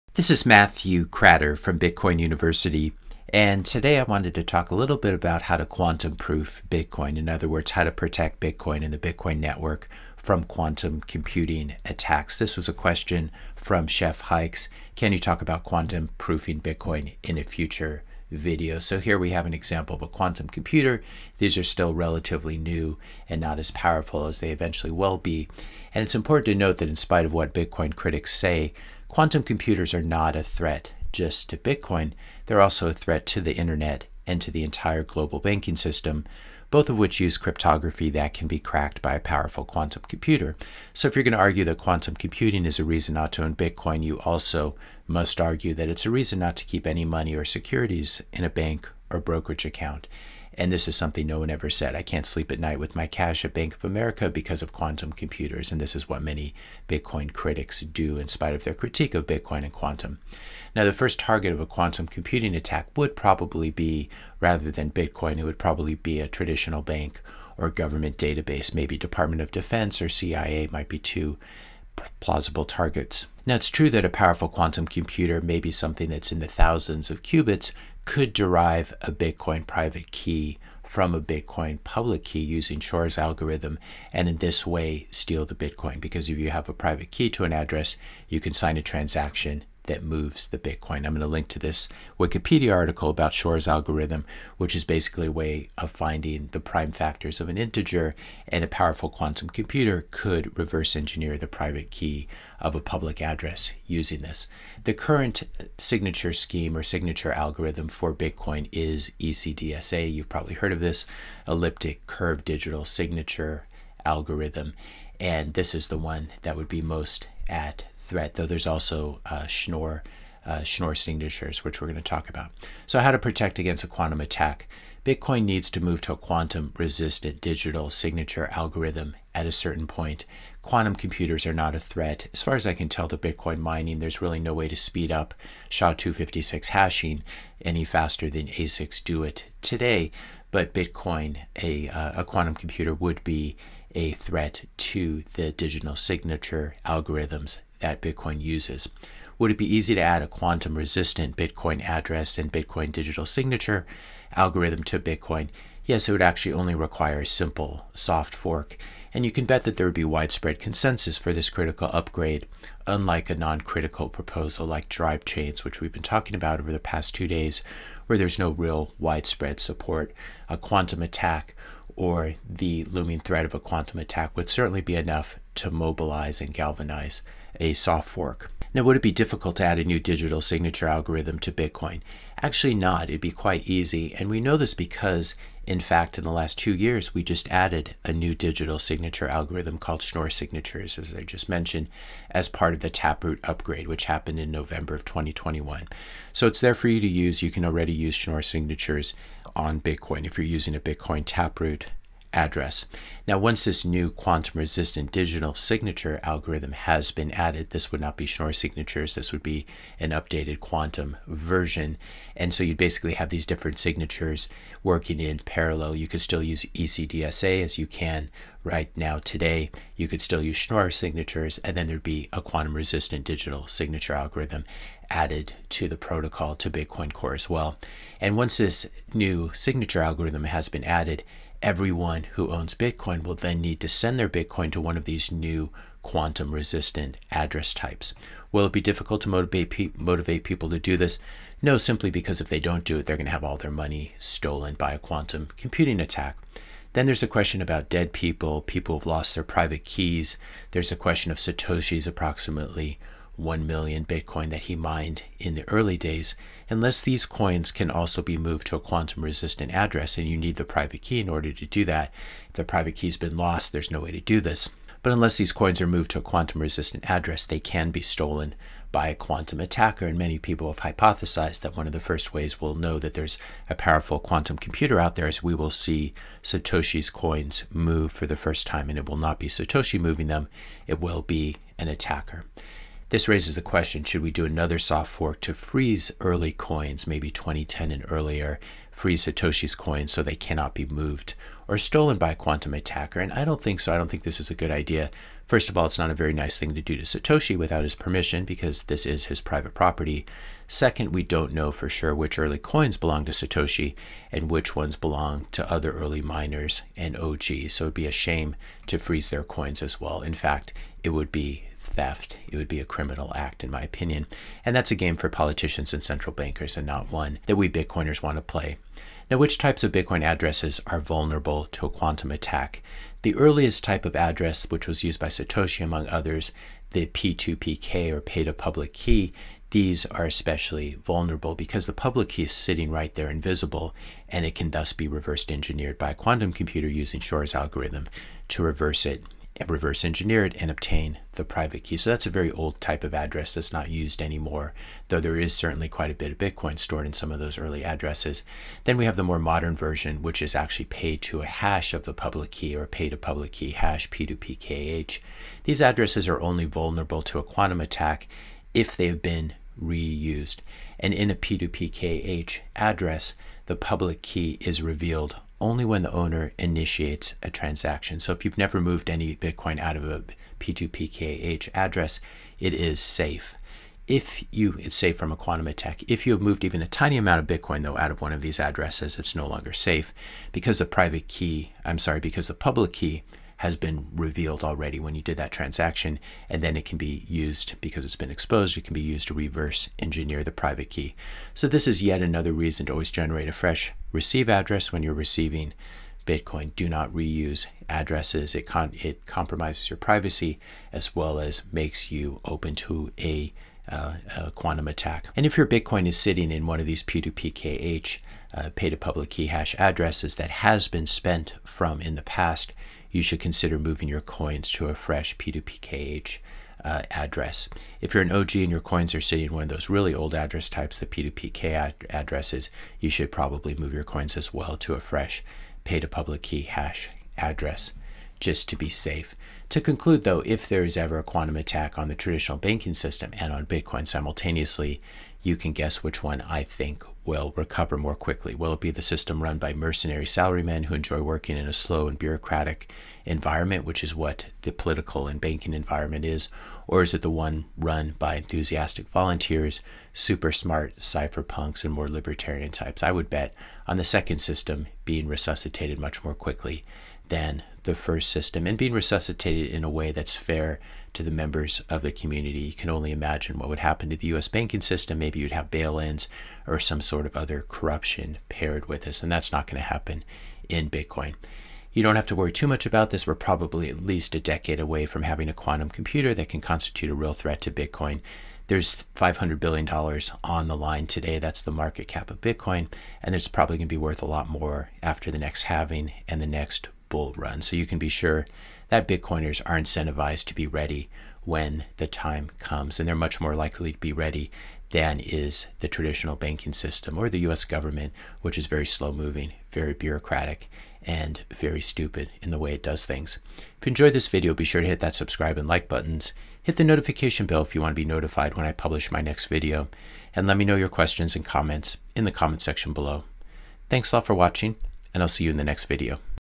How To Quantum-Proof Bitcoin
In this video, I discuss how to quantum-proof Bitcoin by introducing a new quantum-resistant digital signature algorithm.

Quantum computers are just as much a threat (and maybe more) to the government and the traditional banking system, which tend to be much more slower-moving and bureaucratic.

By keeping your Bitcoin in P2PKH addresses that have not been reused, you can help to protect your own coins from a quantum attacker.